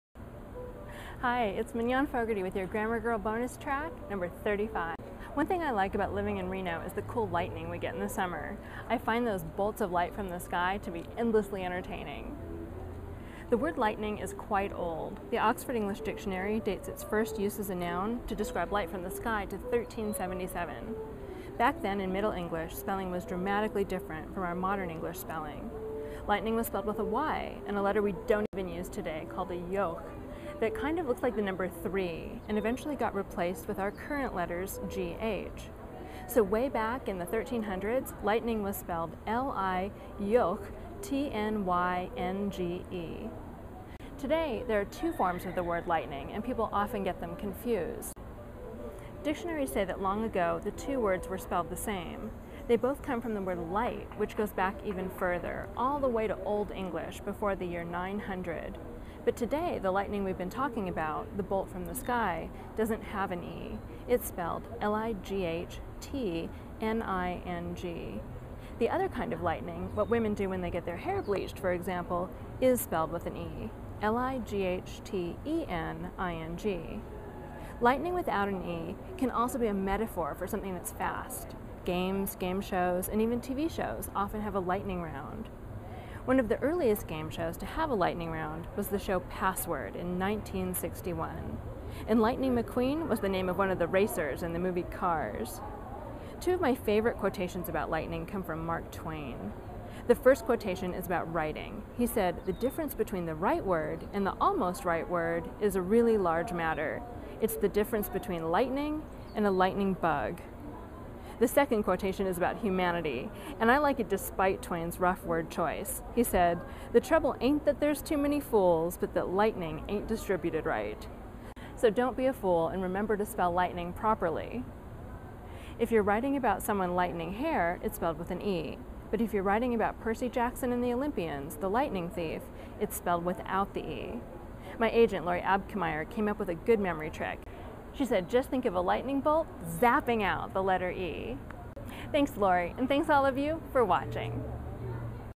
"Lightning" Versus "Lightening" with Grammar Girl, Mignon Fogarty
Should you use "lightning" or "lightening"? Quick and Dirty Tips presents " 'Lightning' Versus 'Lightening' " with Grammar Girl, Mignon Fogarty.  In this episode, learn the history of the two words, their differences, when to use each one properly, and a good trick to remember the difference by.